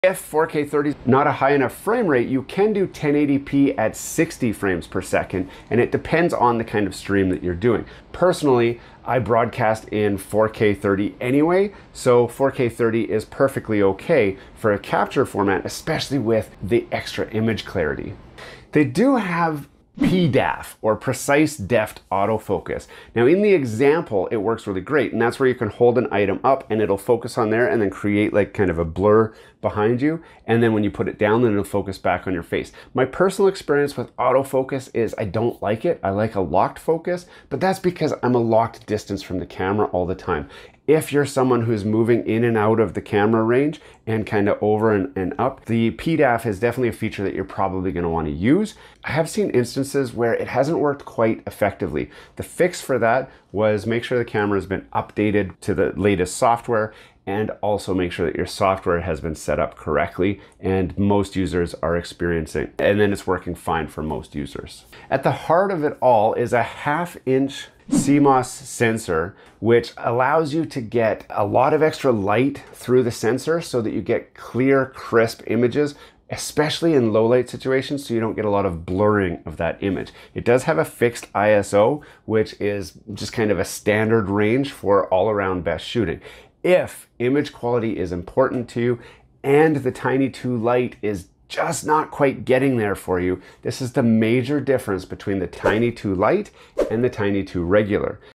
4K vs 1080p: The Ultimate Streaming Frame Rate Battle
Discover the differences between 4K at 30fps and 1080p at 60fps for streaming. We explore autofocus features, sensor capabilities, and how to achieve stunning image quality, even in low light. Learn what works best for your broadcasting needs!